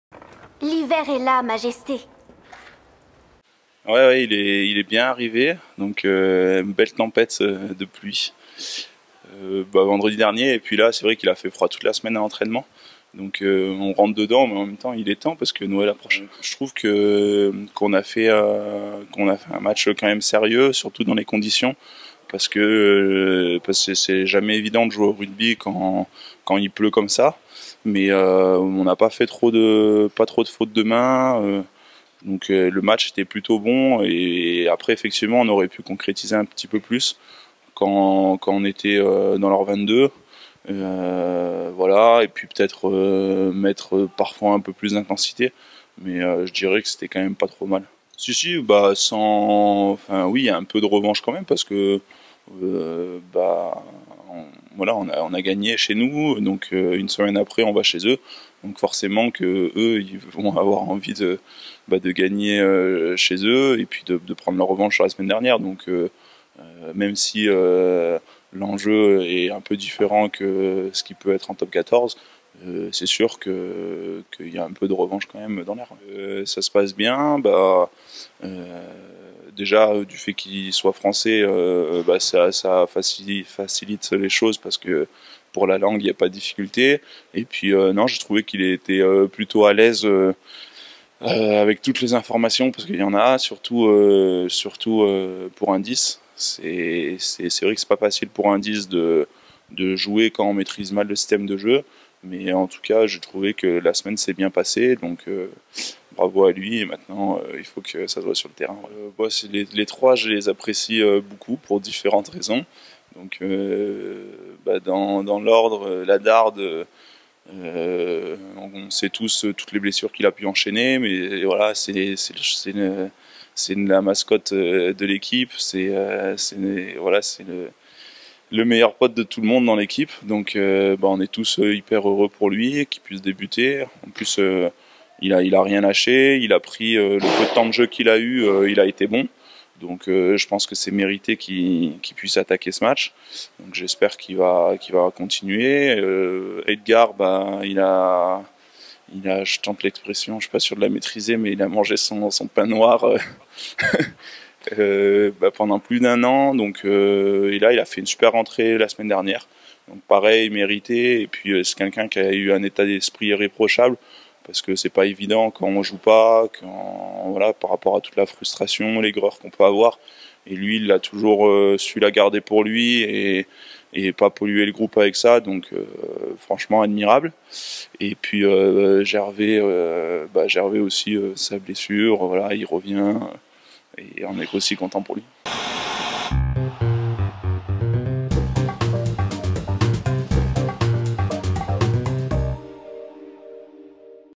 Lucas Dupont : « C’est sûr qu’ils voudront prendre leur revanche »
Lucas Dupont présente la rencontre du FCG à Agen en Challenge Cup, avec la première de Matthieu Ugalde et les titularisations d’Alexandre Dardet, Edgar Tuinukuafe et Gervais Cordin